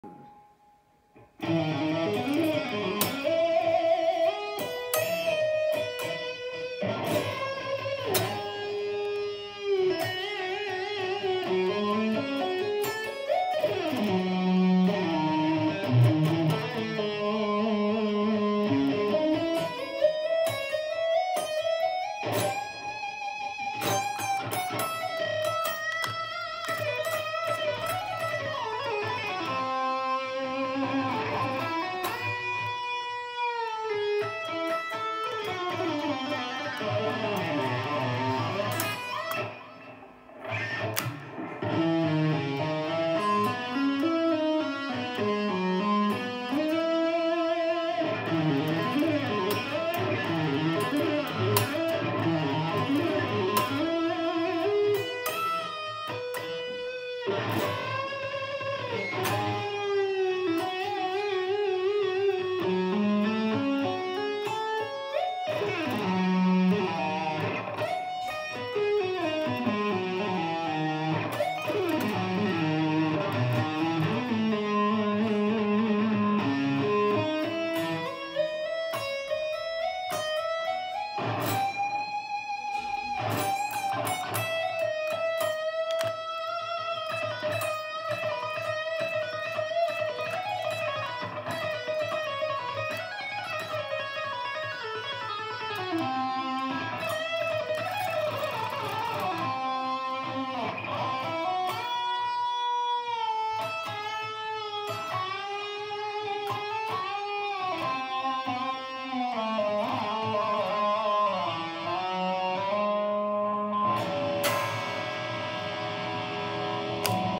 THIS IS WHAT HAPPENS WHEN U PRACTICE NON STOP FOR 30 YEARS !
Stay Updated! 🔔

Thanks so much for the love u showered for our song and the guitar parts !
this time i tried my best to keep the solo as simple as i could for people to cover and learn !
here is the solo in full tempo and then i break it into chewable pieces !
HAVE FUN AND THANK U ONCE AGAIN !